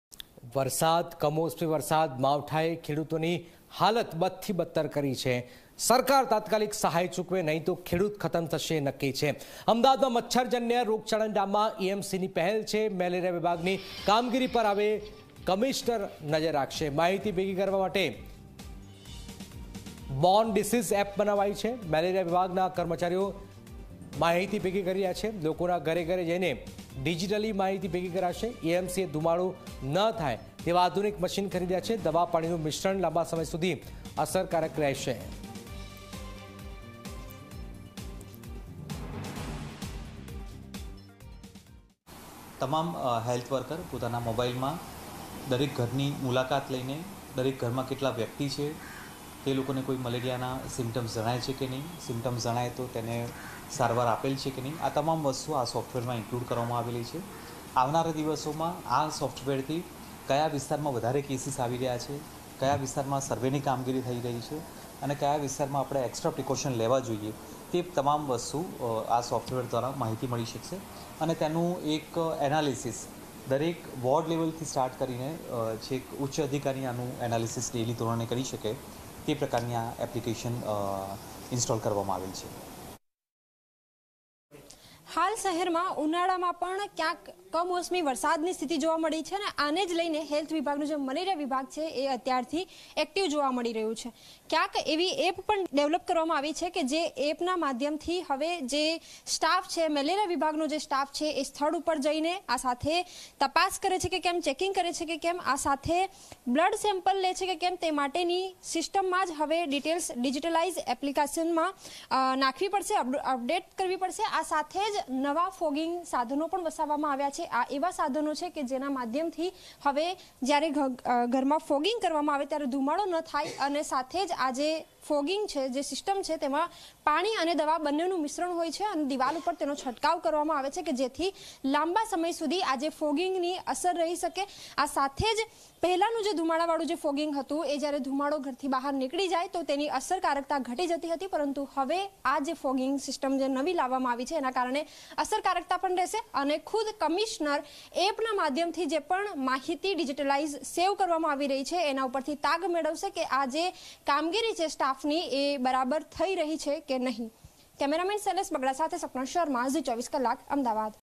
AMC develops application to track record of water,air-borne diseases in the city | Zee News
અમદાવાદમાં મચ્છરજન્ય રોગચાળાને ડામવા AMCની પહેલ ---
મેલેરિયા વિભાગની કામગીરી પર હવે કમિશનર રાખશે નજર  ---
માહિતી ભેગી કરવા માટે બોર્ન ડિસીઝ એપ બનાવવામાં આવી ---
મેલેરિયા વિભાગના કર્મચારીઓ ભેગી કરશે માહિતી ---
લોકોના ઘરે ઘરે જઈને ડીજીટલી માહિતી ભેગી કરાશે ---
AMCએ ધૂમાડો ન થયા તેવા આધુનિક મશીન ખરીદ્યા ---
દવા-પાણીનું મિશ્રણ લાંબા સમય સુધી અસરકારક રહેશે ---